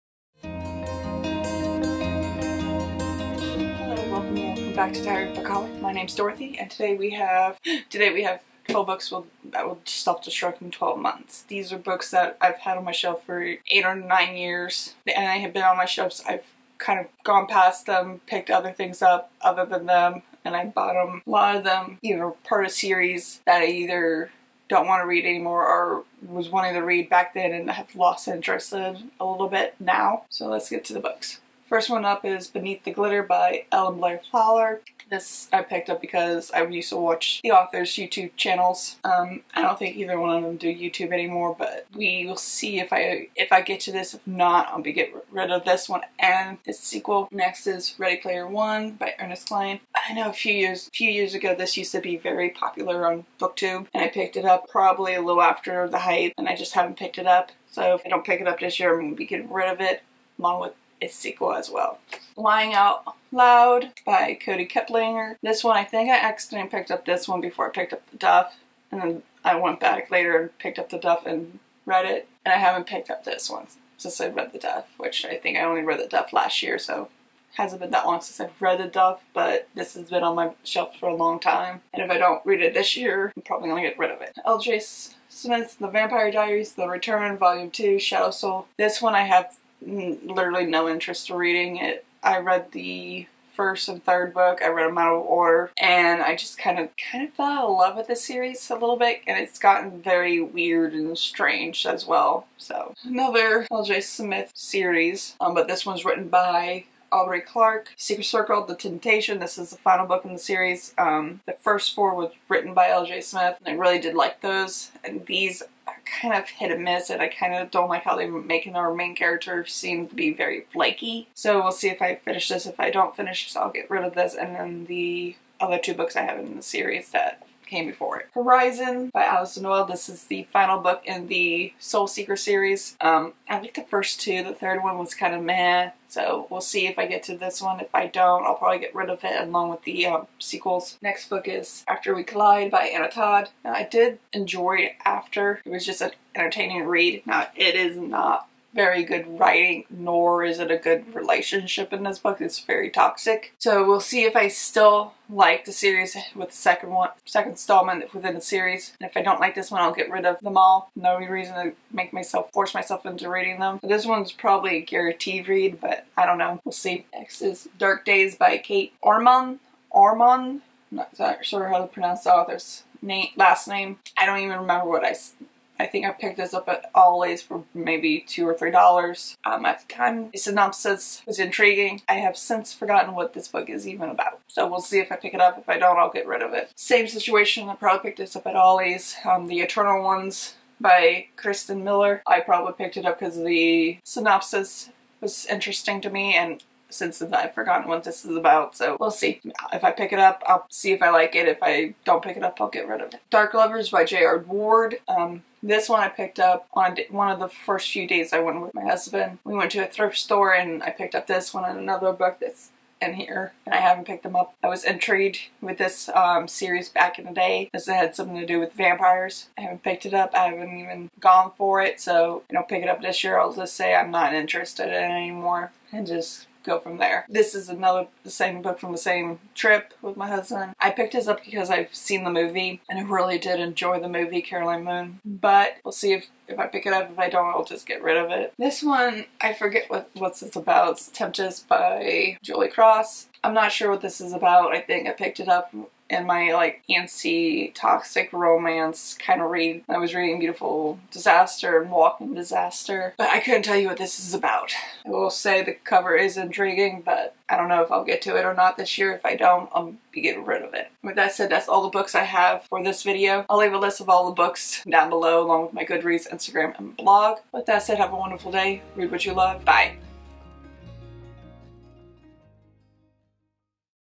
12 books that will self destruct in 12 months
Read Me!
I forgot to mention in the video i got this idea from @BeccaandTheBooks

books mentioned
Tempest by Julie Cross
Carolina Moon by Nora Roberts
Dark Lover by J.R. Ward
After We Collided by Anna Todd
Horizon by Alyson Noel
The Secret Circle: The Temptation by L.J. Smith
Shadow Souls  by L.J. Smith
Lying Out Loud by Kody Keplinger
Ready Player One by Ernest Cline
Beneath the Glitter by Elle Fowler, Blair Fowler
Dark Days by Kate Ormand
The Eternal Ones by Kirsten Miller